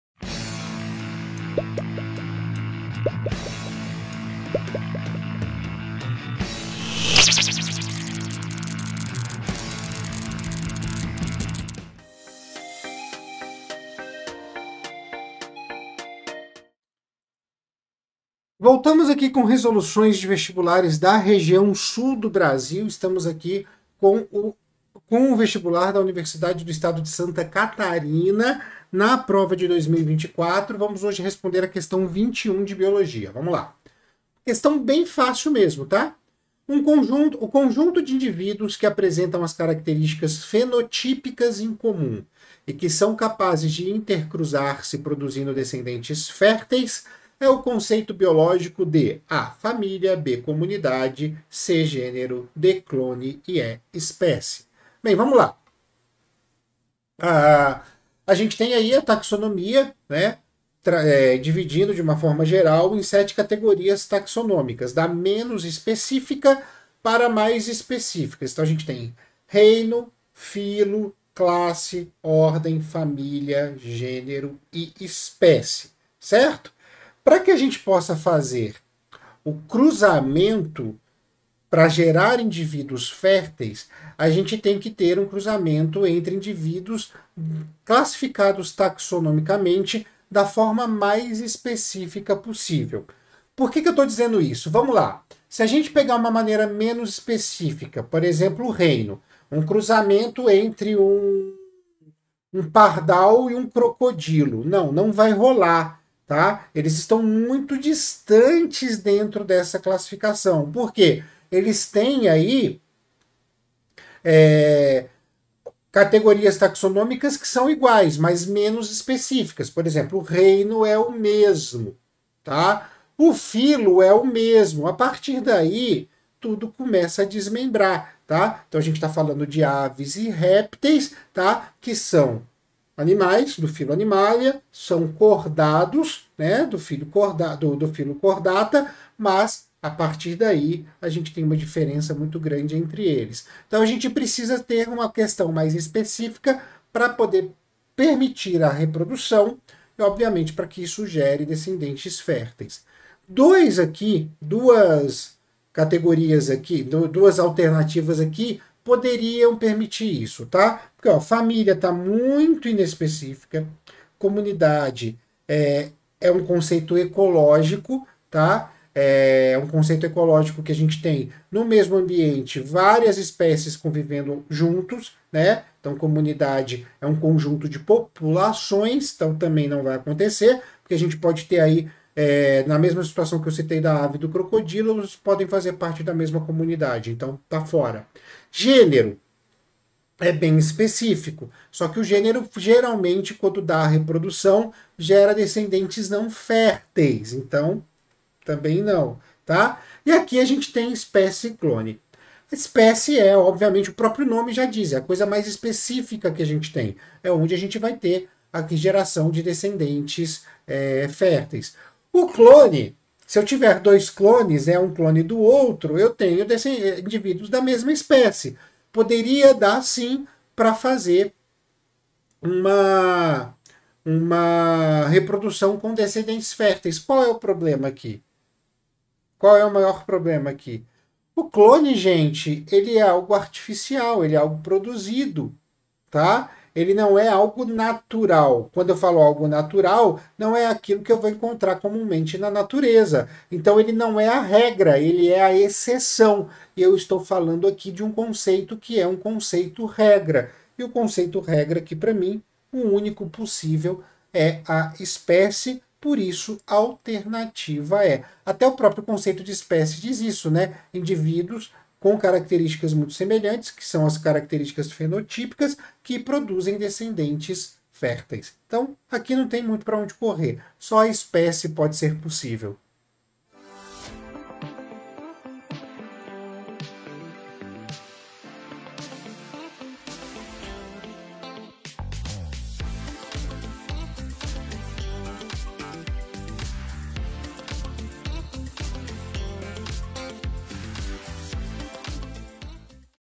RESOLVENDO QUESTÕES - REGIÃO SUL - UDESC 2024 - Questão 21
FRAGMENTO DA QUESTÃO:

O conjunto de indivíduos que apresentam características fenotípicas em comum e que são capazes de intercruzarem-se, produzindo descedentes férteis, é o conceito bioloógico de: